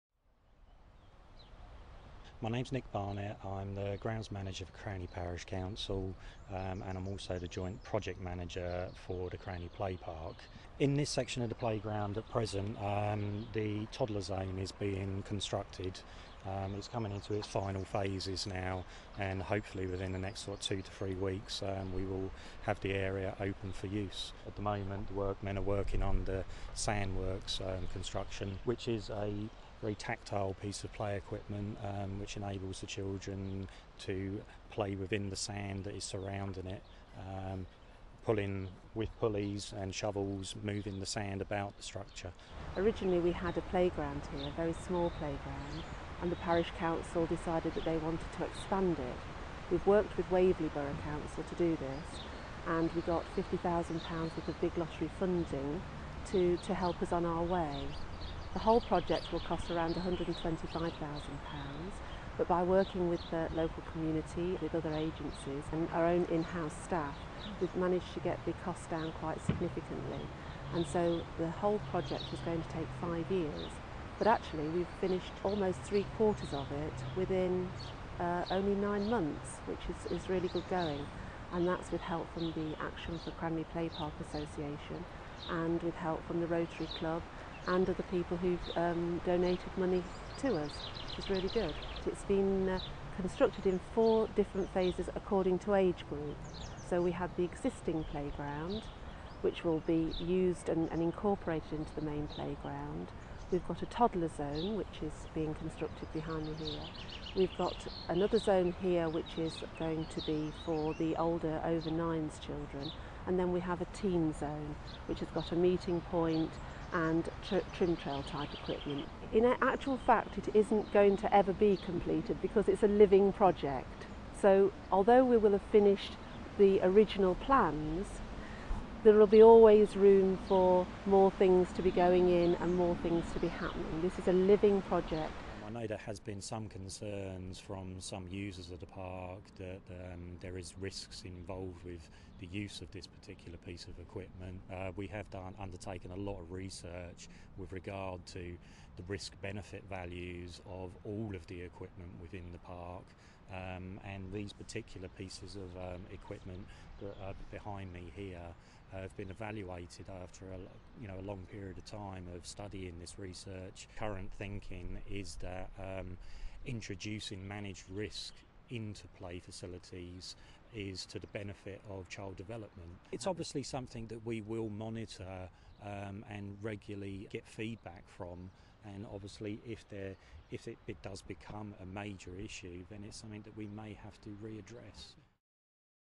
Cranleigh play park revamp
Snoxhall Fields play park revamp runs way ahead of schedule